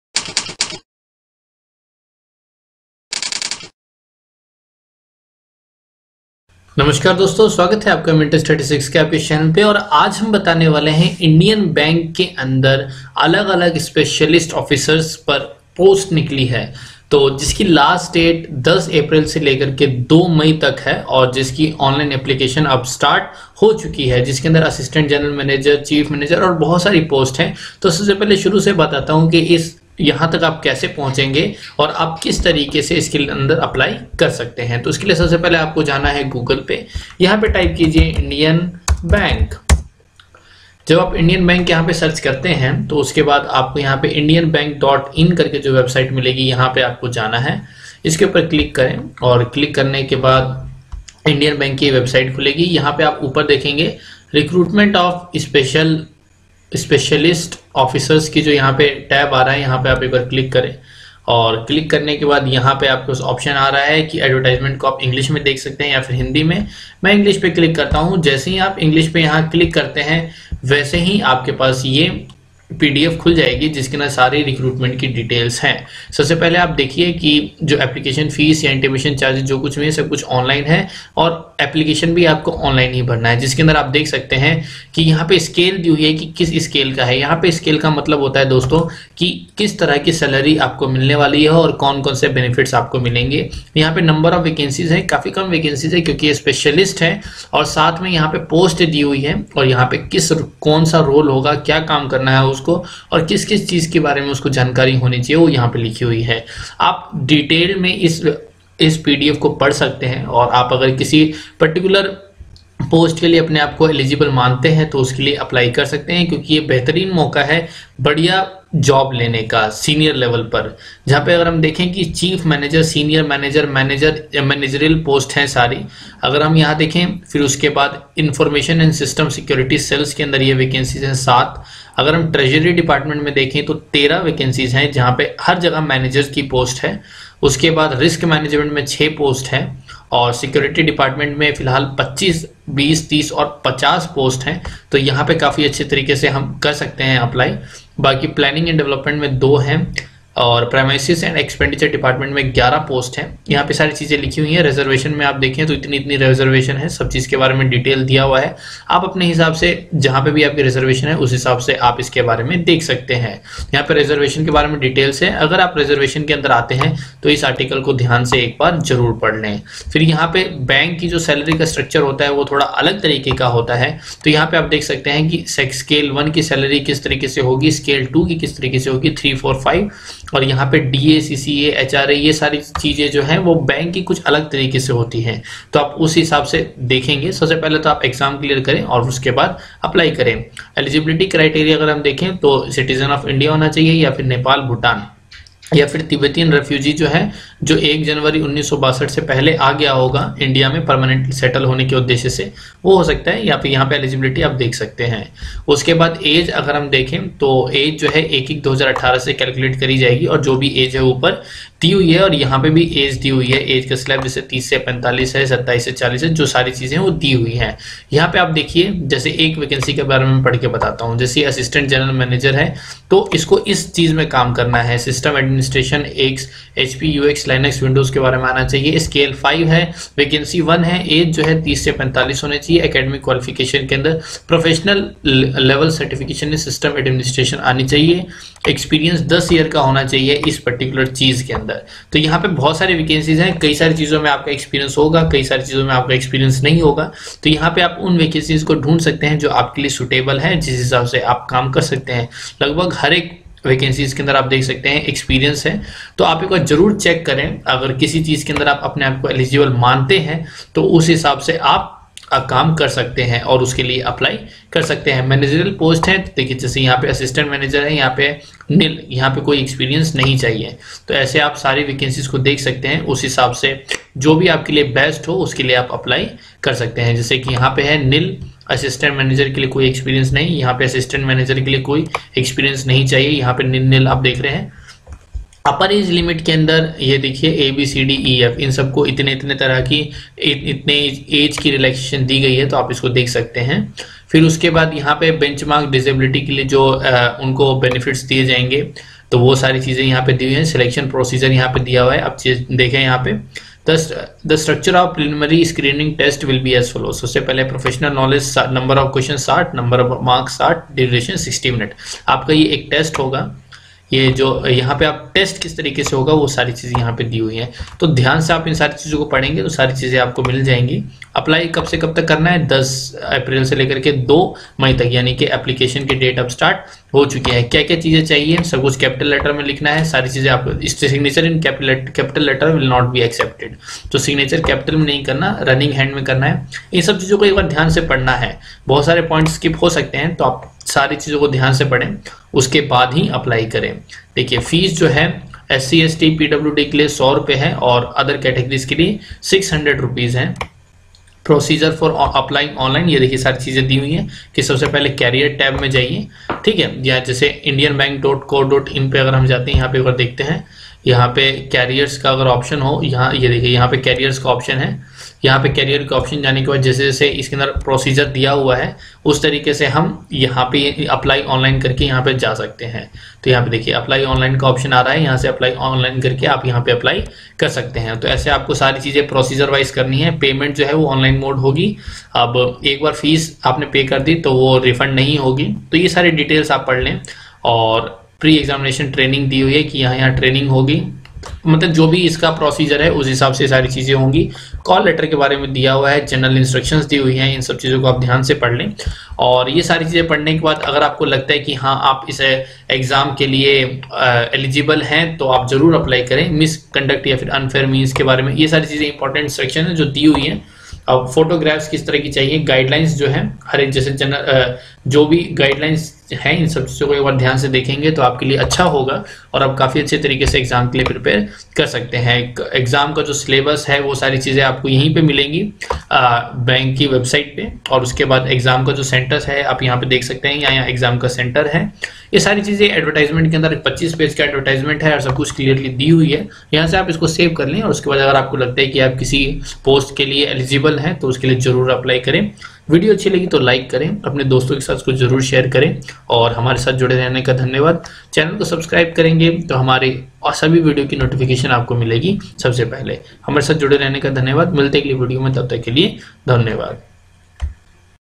Job in Indian Bank for Specialist Officers | Apply before 2 May 2018 | All Details
Know about Indian Bank Job for Specialist Manager. Know about the vacancy and the process to apply for the job.